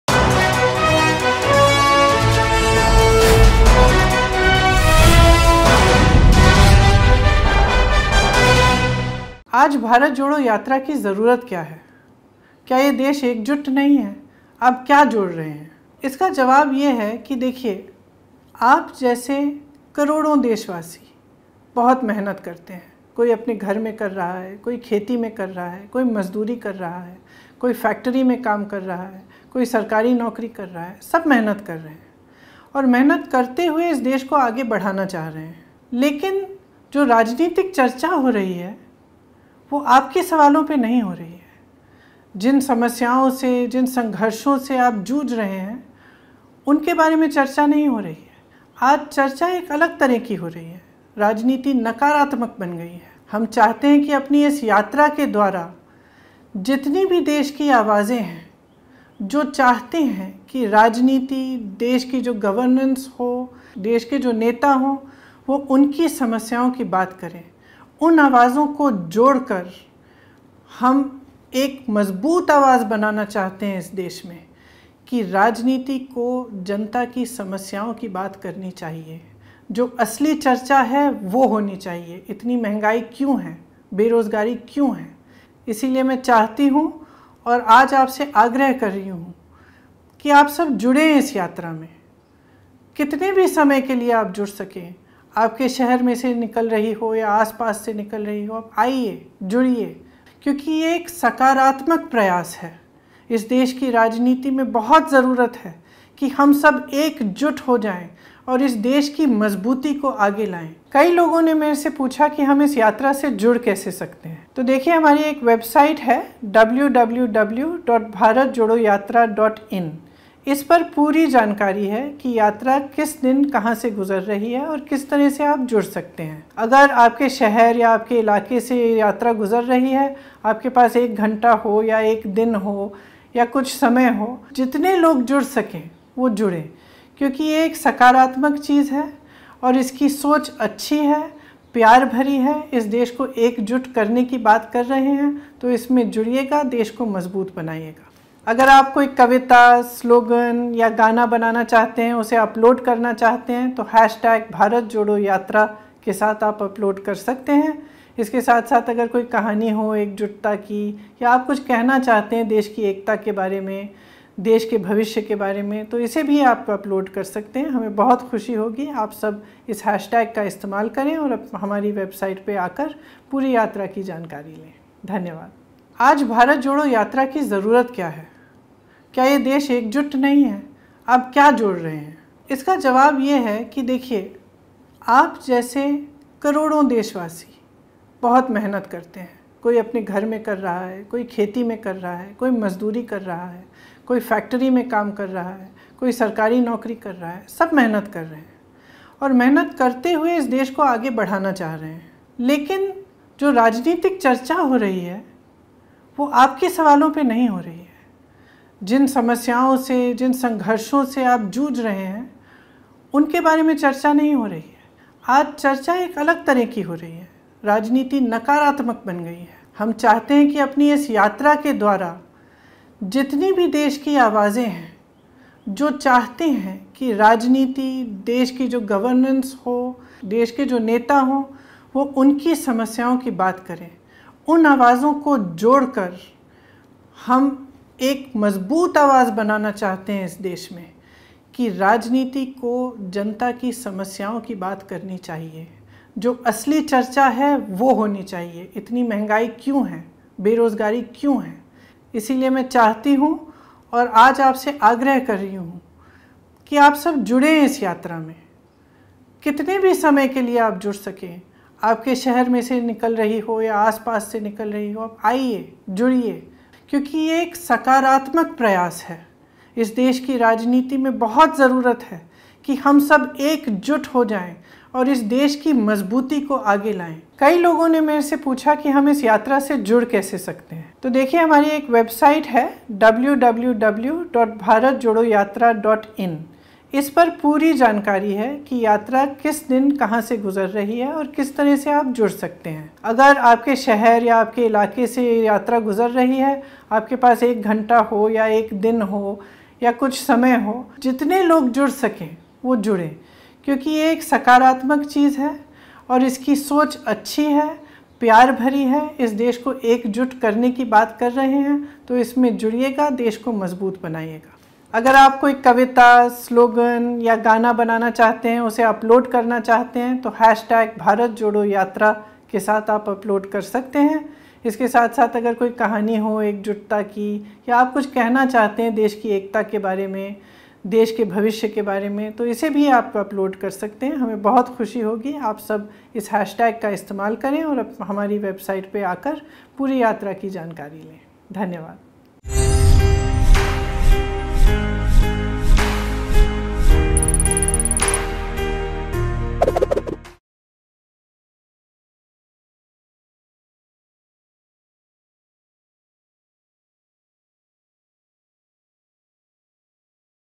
Priyanka Gandhi Appeals People To Participate In Rahul Gandhi's Bharat Jodo Yatra | Mango News
Priyanka Gandhi Appeals People To Participate In Rahul Gandhi's Bharat Jodo Yatra.